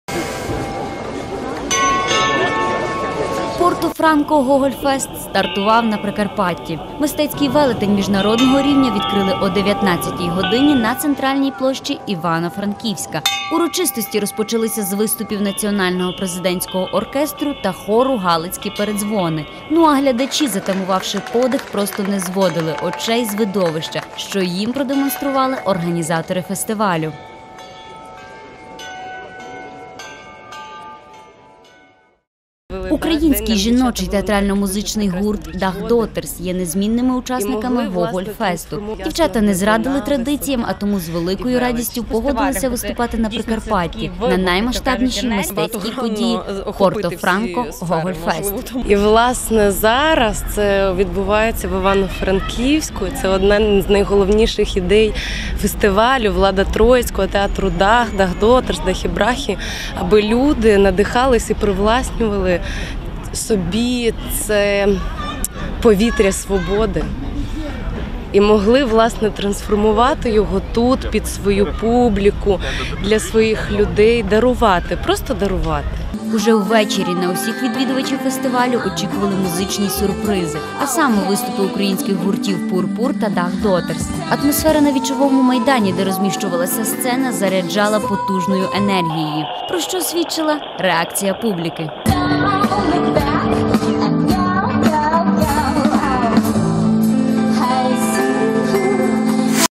Відкриття ПортоФранкоГогольФесту
ПортоФранкоГогольФест стартував на Прикарпатті. Мистецький велетень міжнародного рівня відкрився о 19.00 на центральній площі Івано-Франківська. Урочистості розпочалися з виступів національного президентського оркестру та хору "Галицькі передзвони".